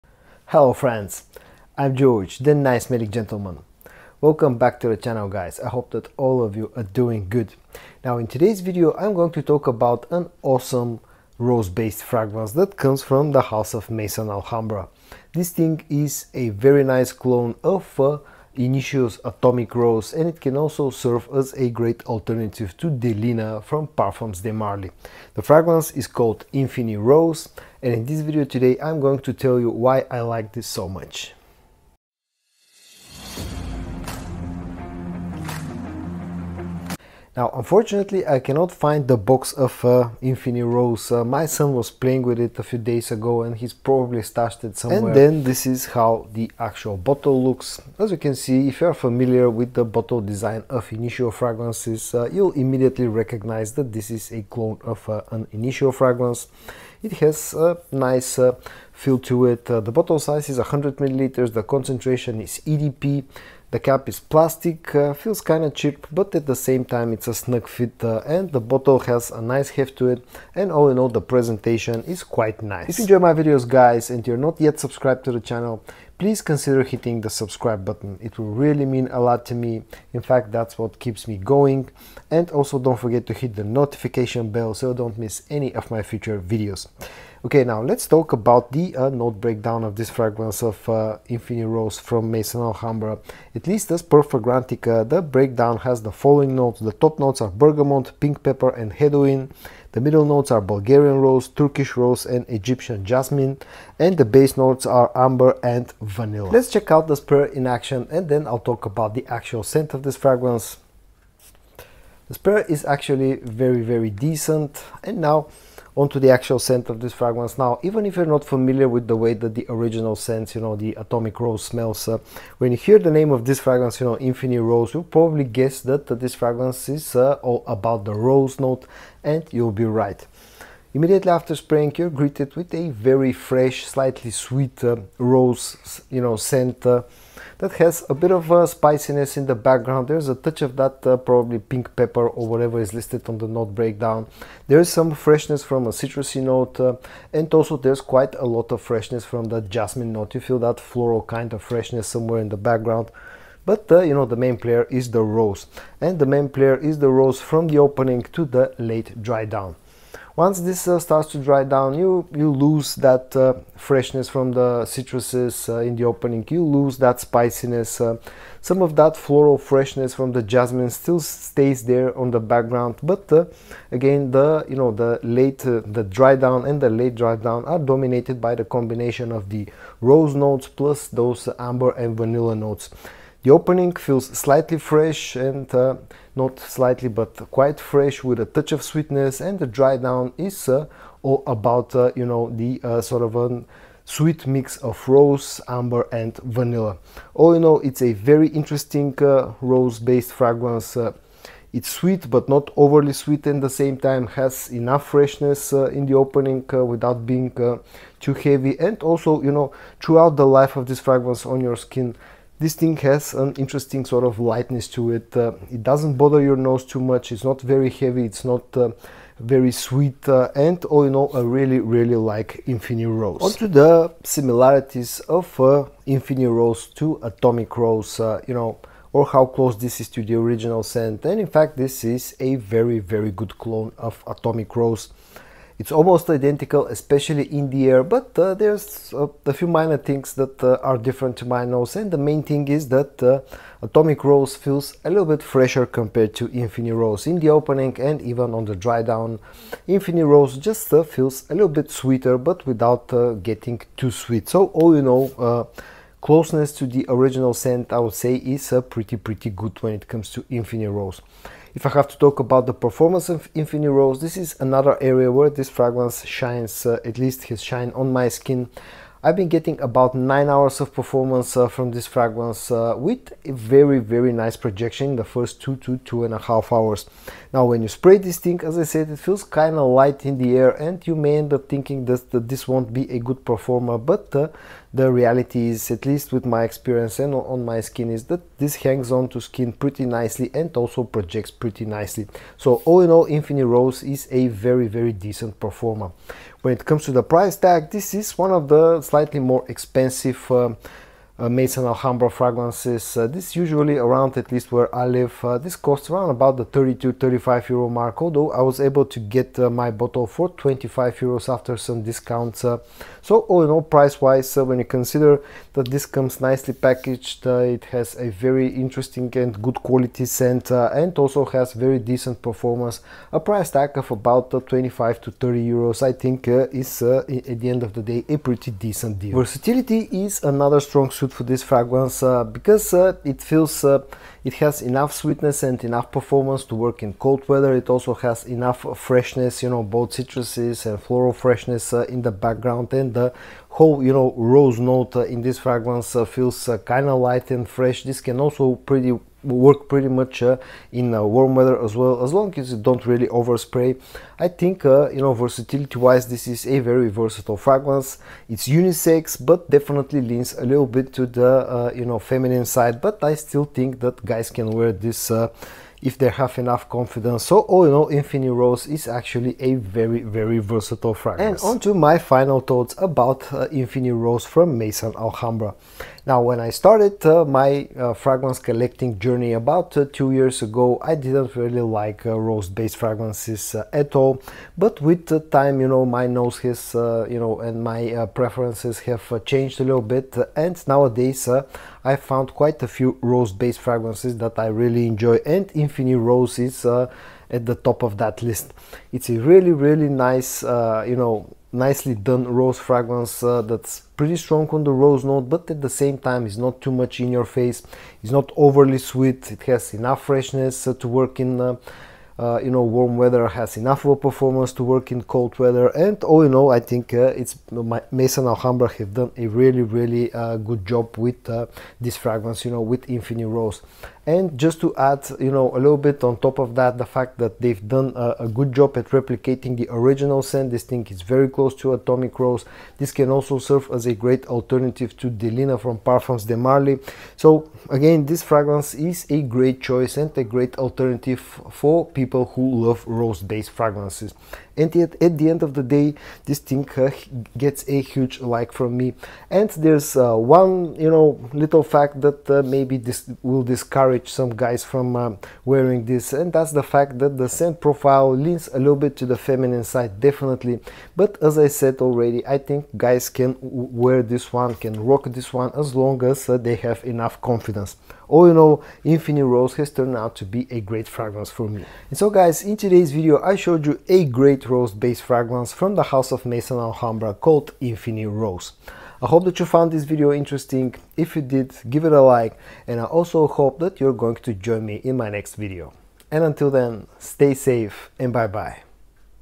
An AWESOME Rose Fragrance From Maison Alhambra the Infini Rose! | A Great Initio Atomic Rose Clone!
If you enjoy my content you can show your support by hitting the "Thanks" button :)

My review of an awesome rose fragrance from Maison Alhambra - the Infini Rose! This can also be used as an alternative to PDM Delina